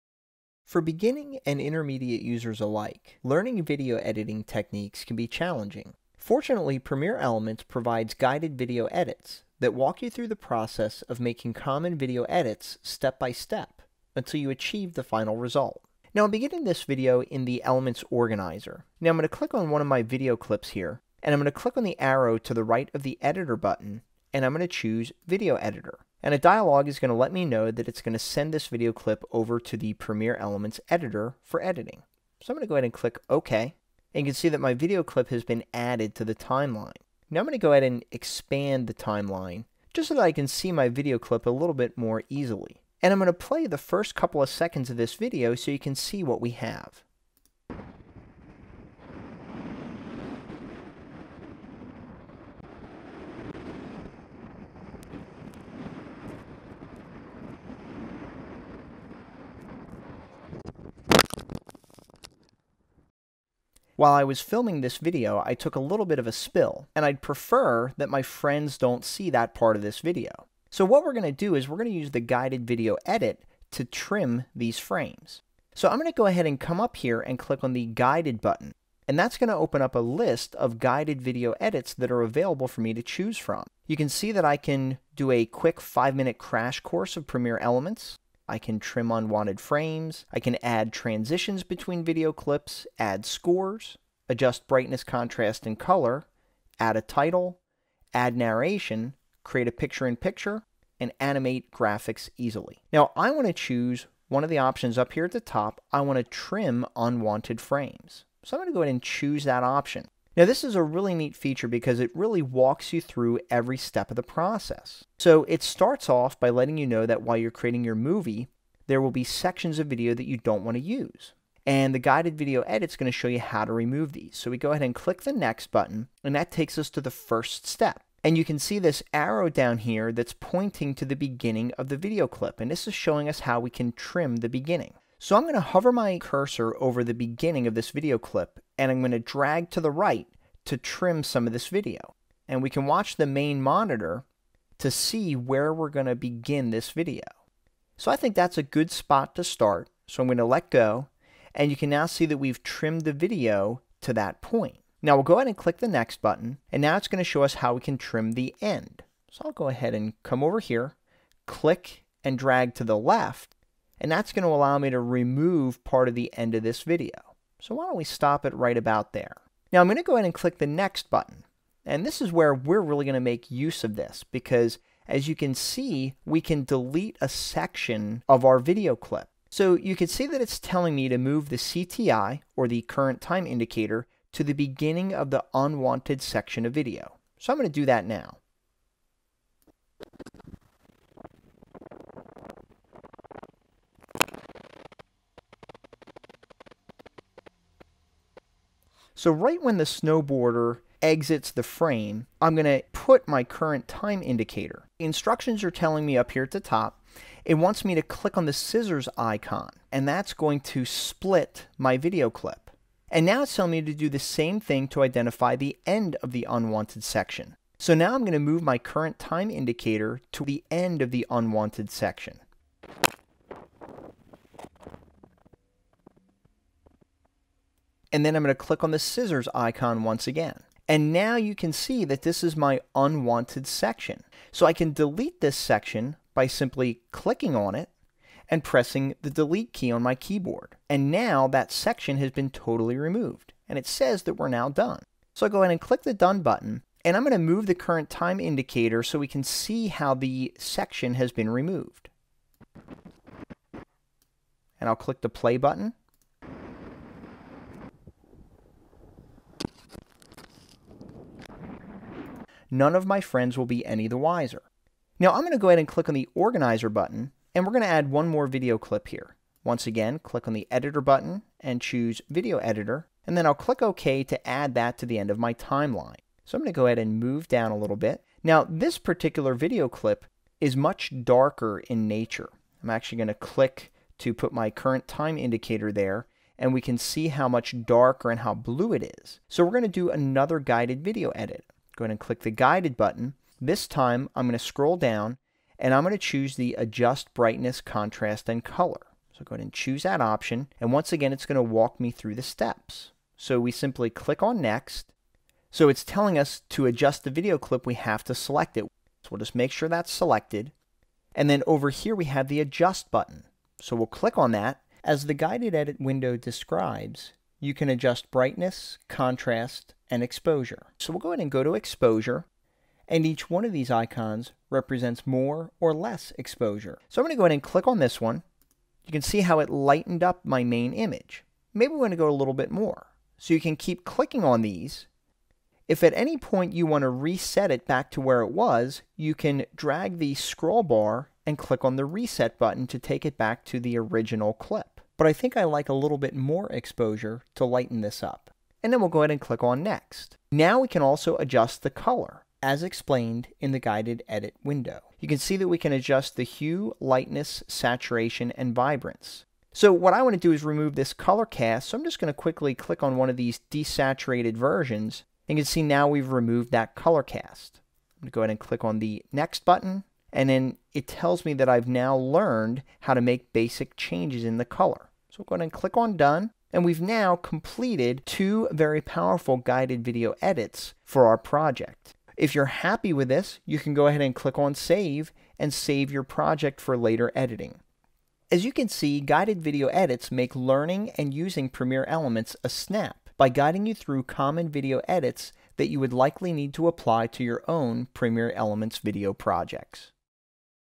Using Guided Edits in Premiere Elements 12
Use Guided Edits to learn advanced video editing techniques with built-in tutorials.